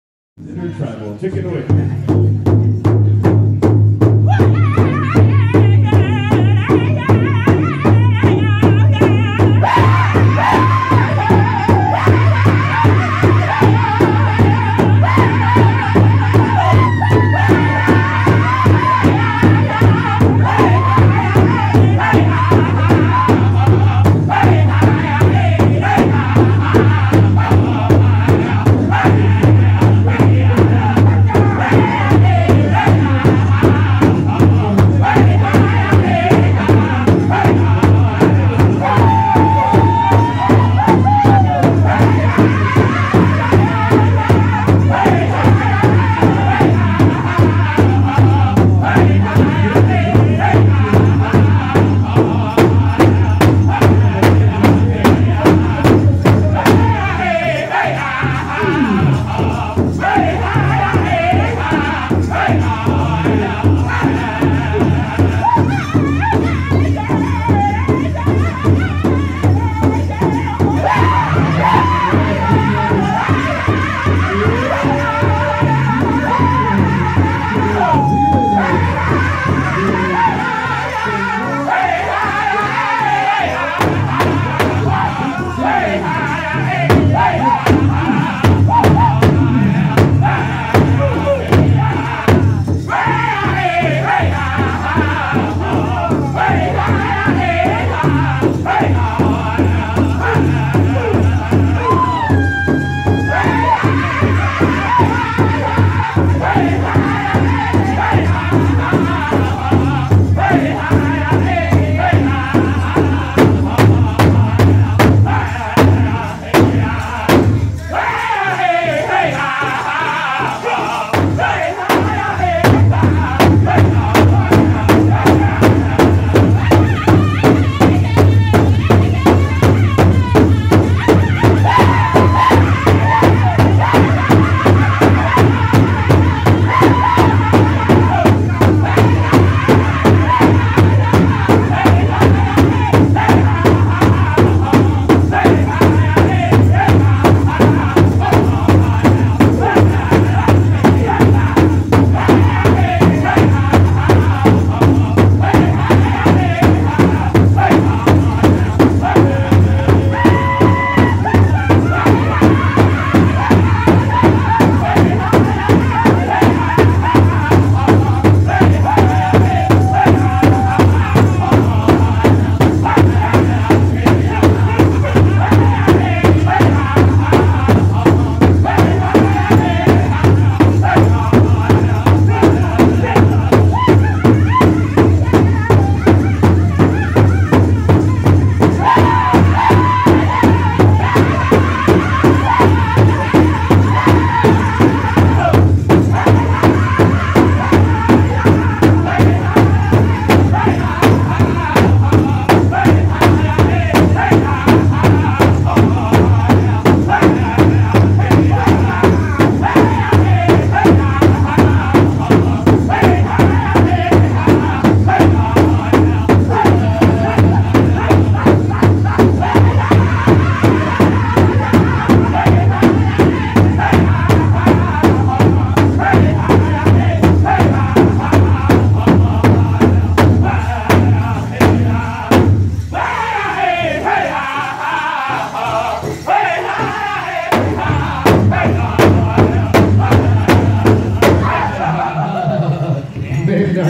Midnite Express Sat. Afternoon Mt. Pleasant Powwow 2016
Mnx back in Lake Mitt.
1st contest song Saturday afternoon in Mount Pleasant.